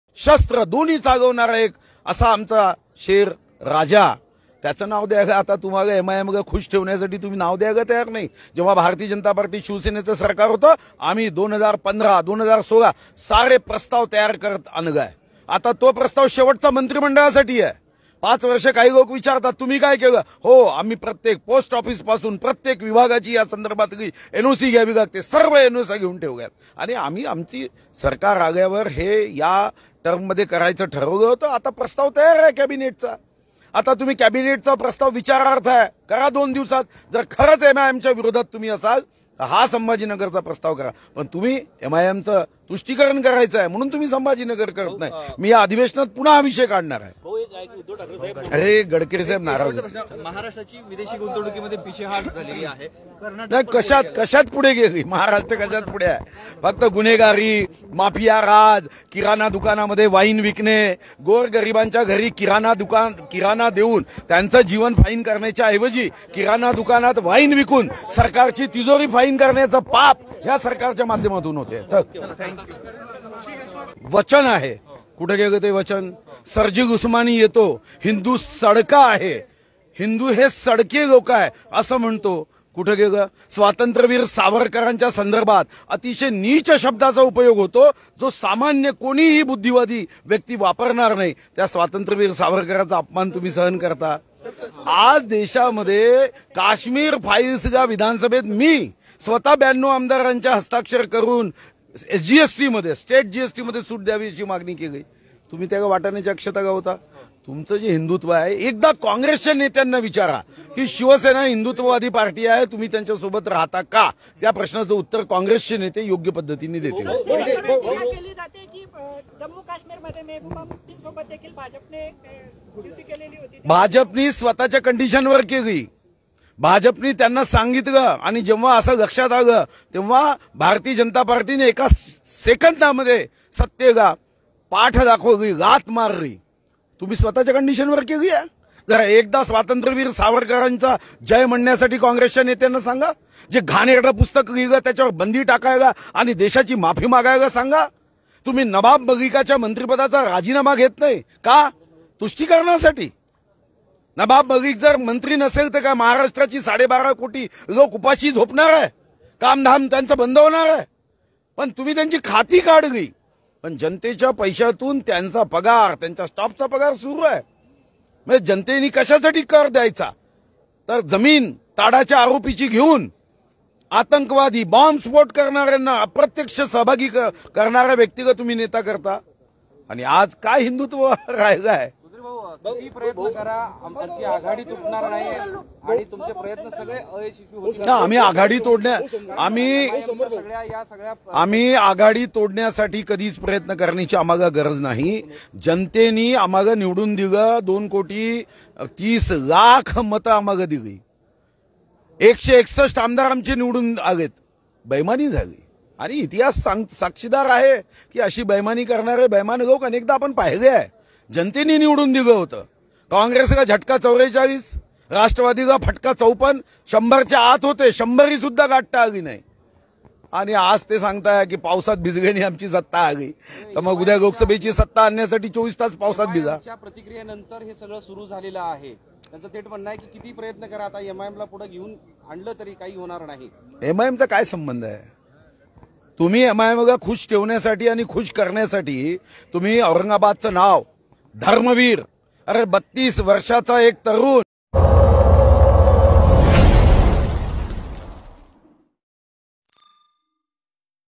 जळगाव : एम आय एम ला खुश ठेवण्यासाठी छत्रपतींचे नाव औरंगाबादला द्यायला तयार नाही -सुधीर मुनगंटीवार
@rajkiyadarshan जळगाव : एम आय एम ला खुश ठेवण्यासाठी छत्रपतींचे नाव औरंगाबादला द्यायला तयार नाही -सुधीर मुनगंटीवार

ANC :  जम्मू-काश्मीरमध्ये भाजपने सत्तेला एका सेकंदामध्ये लाथ मारली, तुम्ही स्वतःच्या कंडिक्शन वर किती केली आहे एकदा स्वातंत्र्यवीर सावरकर म्हणून जय म्हणायला सांगा कॉंग्रेसच्या नेत्यांना अशी शिवसेनेवर सुधीर मुनगंटीवार यांनी भोसरी टीका आज जामनेर येथे केली आहे .स्वातंत्र्यवीर सावरकर यांच्यावरती घाणेरड्या पुस्तक लिहिलं त्याच्यावर बंदी घालायला सांगा, नवाब मलिक जर महाराष्ट्राचे मंत्री नसतील तर महाराष्ट्रातील लोक काय उपाशी झोपणार आहेत का असा घणाघाती टीका करत सुधीर मुनगंटीवार यांनी जामनेर येथे आज गिरीश महाजन यांच्या कन्येच्या विवाहप्रसंगी आल्यानंतर पत्रकारांशी संवाद साधला.

BYTE :  सुधीर मुनगंटीवार